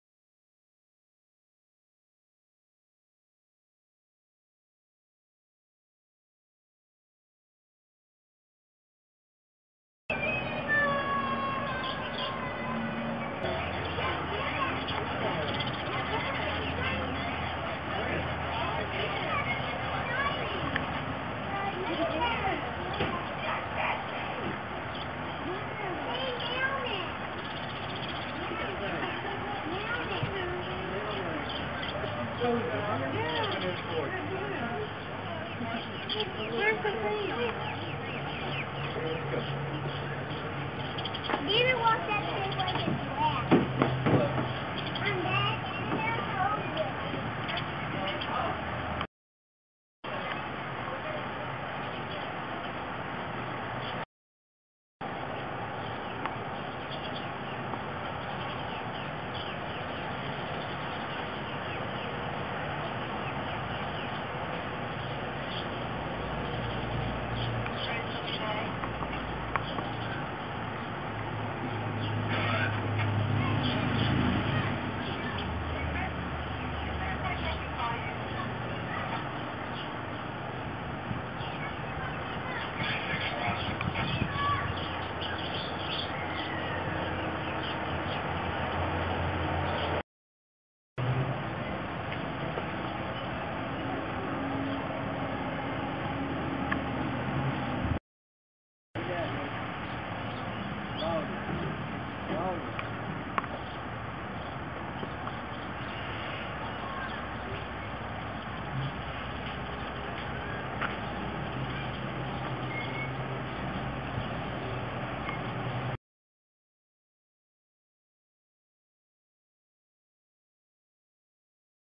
Creative Writing Video -- Reticulated giraffe at the Knoxville Zoo
\After having your class watch this short video about a Reticulated giraffe at the Knoxville Zoo, involve your students in composing either a poem or story.